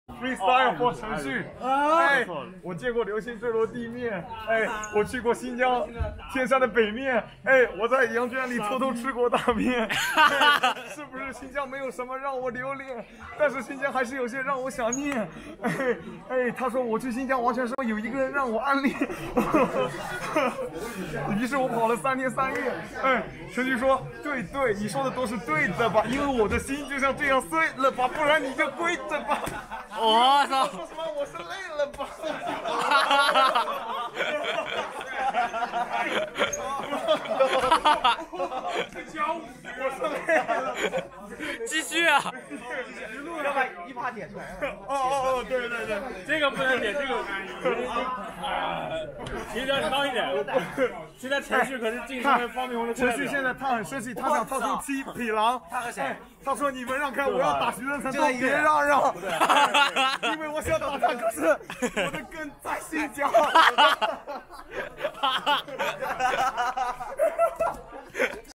Rare Heated School Freestyle! 🔥😱
All the lyrics are improvised！
Language:Chinese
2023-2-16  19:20
From Jiangsu Province Changshu High School, China